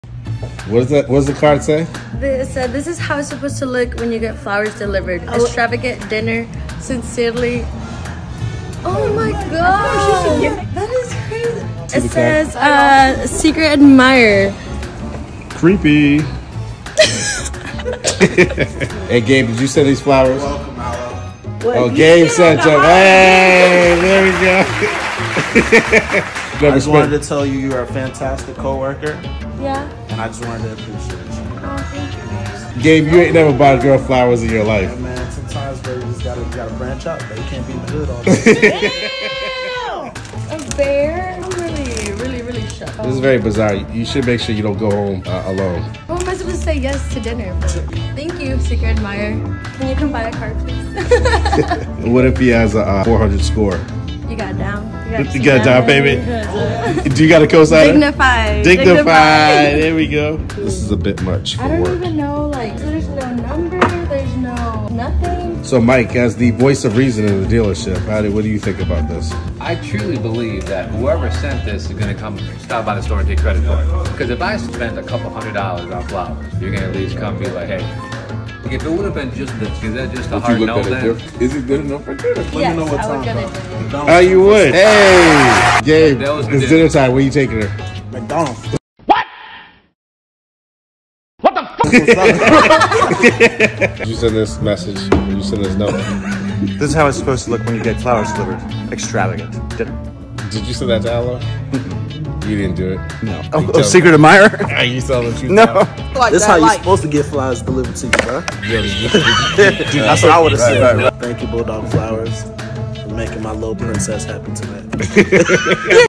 LIFE AT THE DEALERSHIP VLOG #1: ALO GETS ANONYMOUS FLOWERS AT WORK
Alo mysteriously receives 6 bouquets of flowers and a pink teddy bear at the dealership.

Will the secret admirer reveal himself?